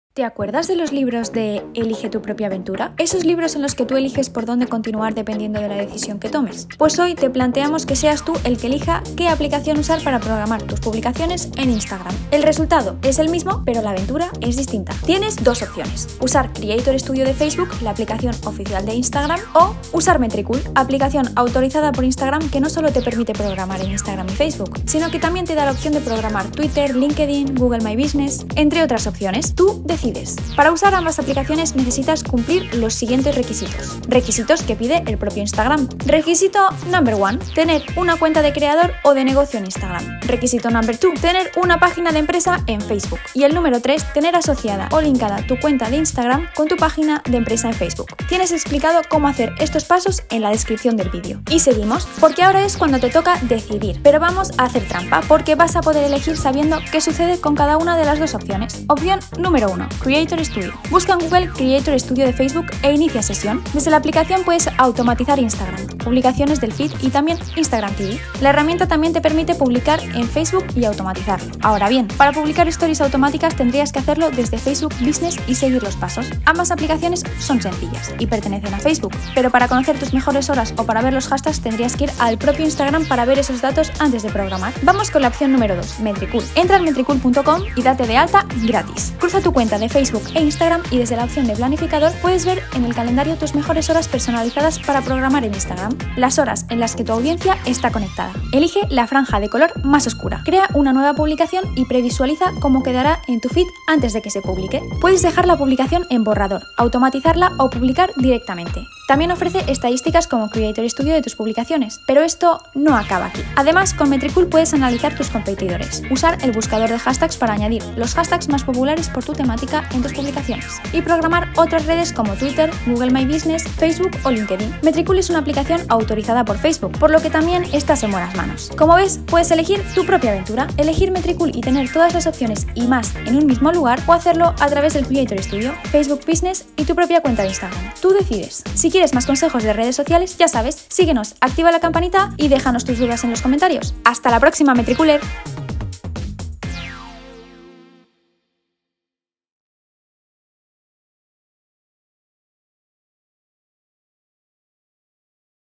APPS para programar INSTAGRAM 📲 [GRATIS]
Estos son los pasos para poder usar Creator Studio o Metricool y programar todas tus publicaciones en Instagram.

00:16 Creator Studio o Metricool
00:32 Requisitos para programar en Instagram
00:59 Opción 1: Creator Studio
01:26 Opción 2: Metricool
01:54 Analiza competidores y hashtags
02:06 Metricool es autorizada por Facebook

Necesitas configurar Instagram como cuenta de creador o empresa y vincularla a una página de Facebook.

3- El paso a paso para vincular Instagram con tu página de Facebook
4- Cómo conectar tu cuenta de Instagram con Metricool
5- Los tipos de perfiles en Instagram, qué puedes y qué no puedes hacer.

Ahora que ya tienes todo lo necesario para usar las apps para programar Instagram tú eliges.
Tienes dos apps gratis.
⭐️ Creator Studio de Facebook
⭐️ Metricool, aplicación autorizada por Facebook

Con ambas puedes automatizar el contenido de Instagram, ahora bien con Metricool la cosa no queda solo en Instagram, también puedes gestionar, programar y automatizar otras redes sociales: Twitter, LinkedIn, Google My Business. Crear informes y obtener analítica de todas tus publicaciones.

Y, para más consejos de marketing o estar en contacto.